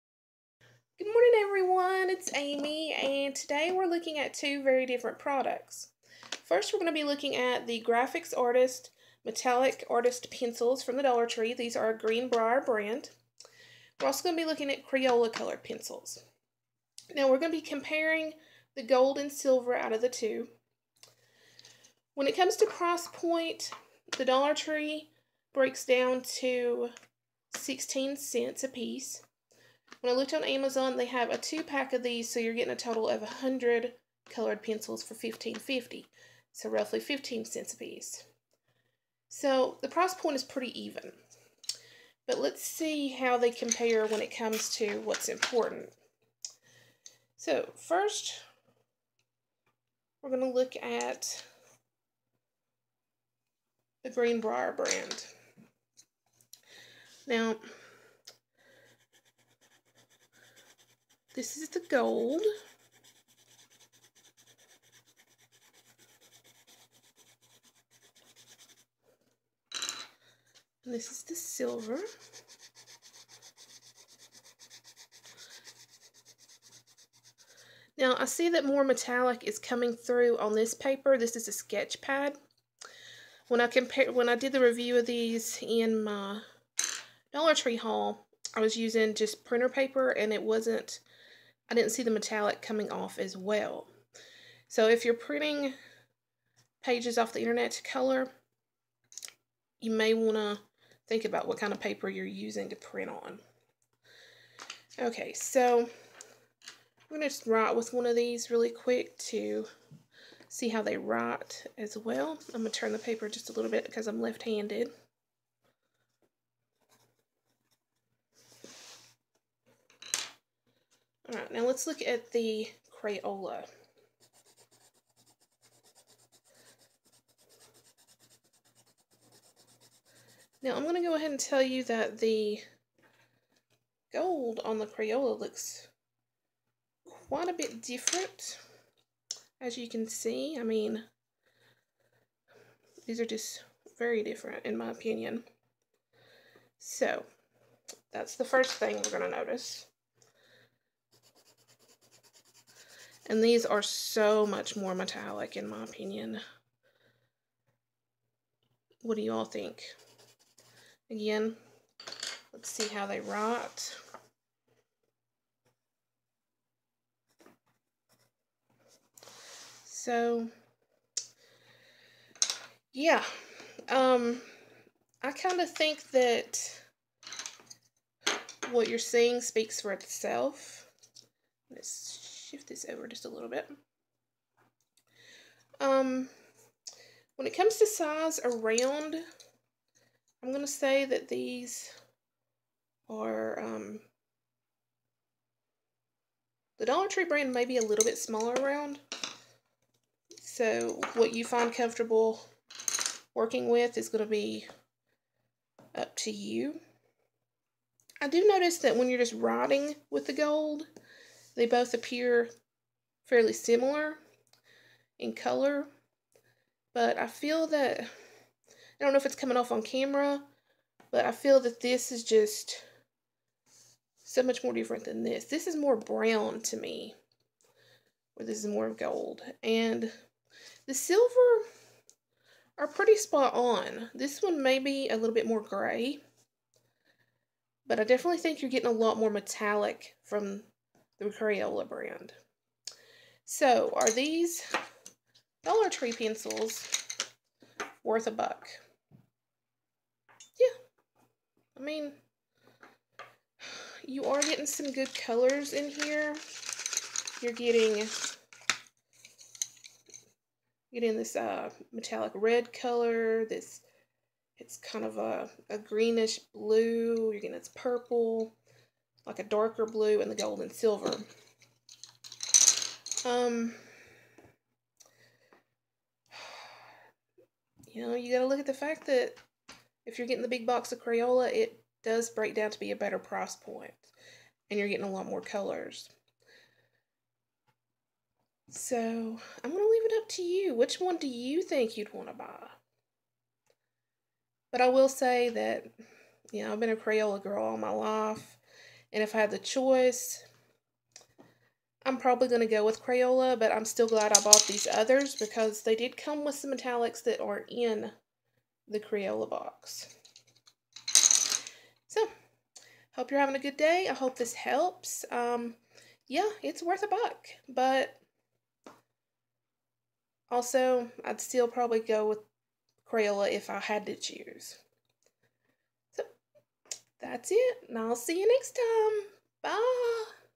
Dollar Tree vs. Crayola: Metallic Colored Pencils
How do the Dollar Tree Graphix Metallic Artist Pencils compare to Crayola brand in price and quality?